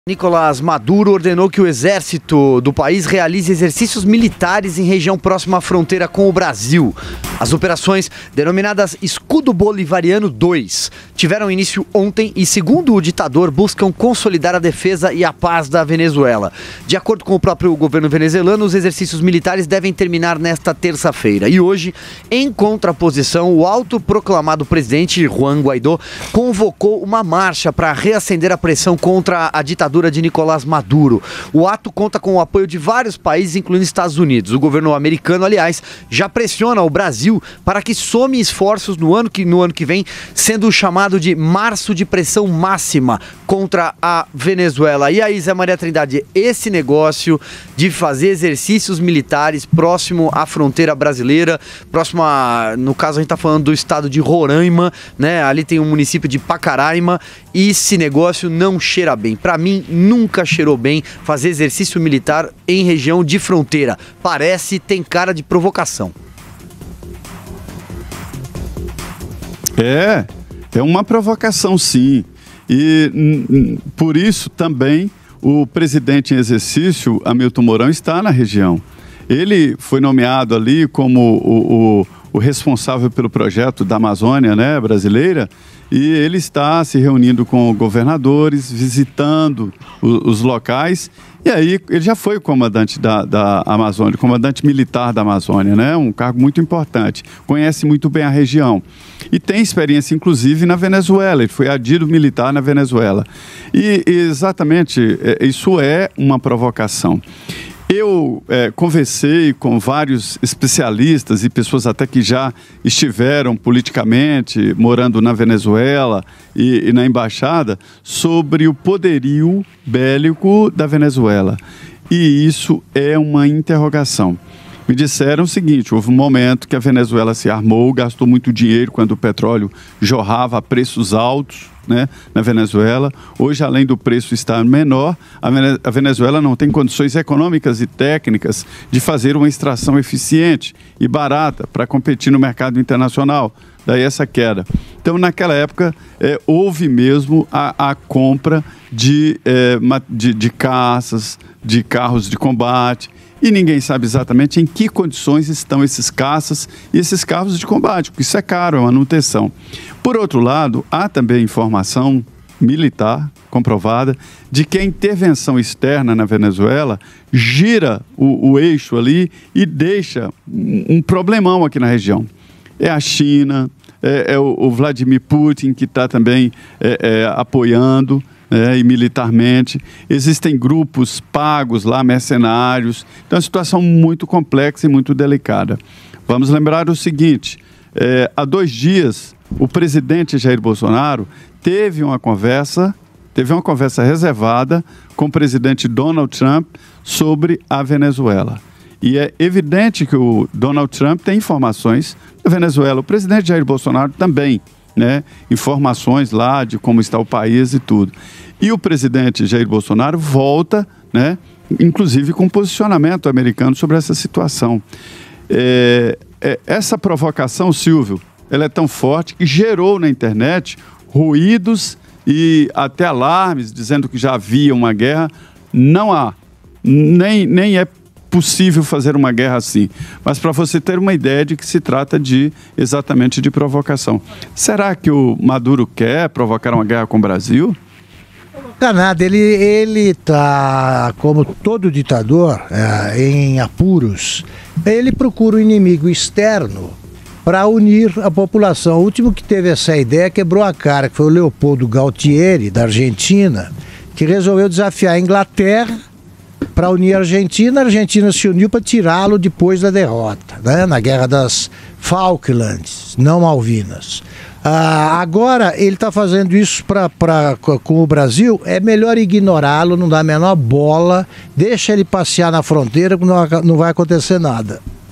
Maduro quer guerra contra o Brasil?
Augusto Nunes, José Maria Trindade e Silvio Navarro comentam os exercícios militares ordenados pelo ditador Nicolás Maduro na fronteira da Venezuela com o Brasil.